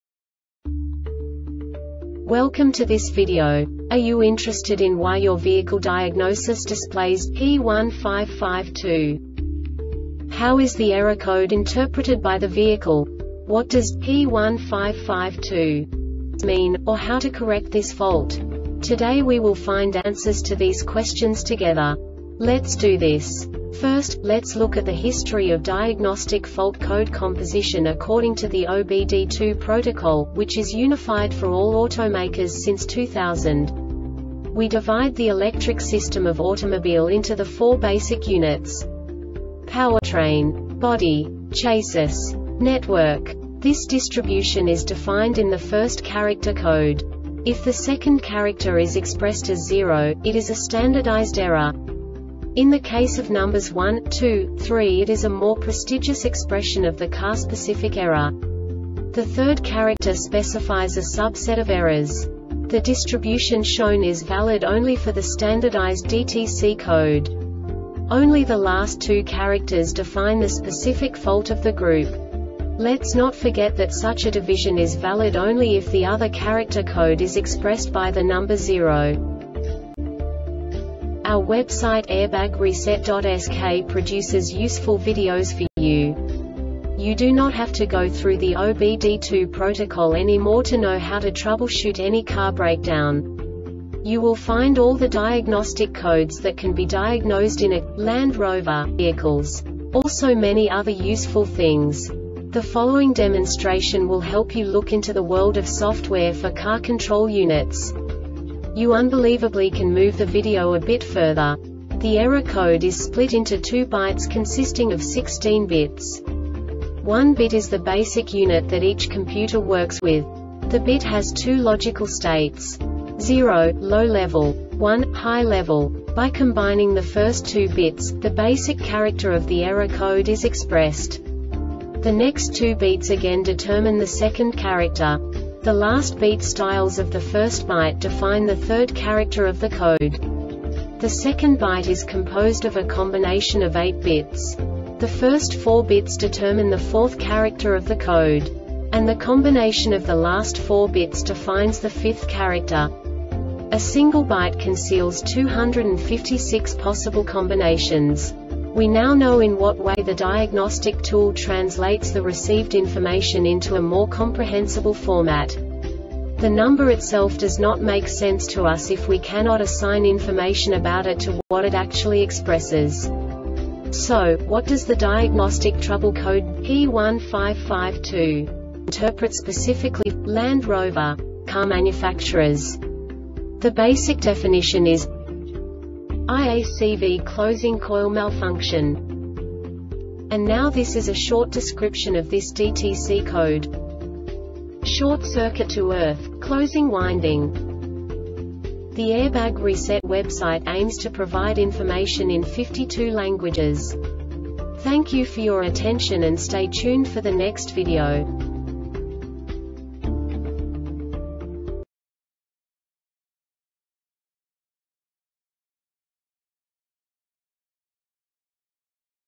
DTC Landrover P1552 Short Explanation
Contents:
0:21 Basic DTC analysis according to OBD2 protocol standard.
1:48 Insight into programming
2:58 A diagnostically understandable description of the Diagnostic Trouble Code
3:05 Basic error definition

Make
Land Rover
Code
P1552
Definition
IACV closing coil malfunction
Description
Short circuit to earth - closing winding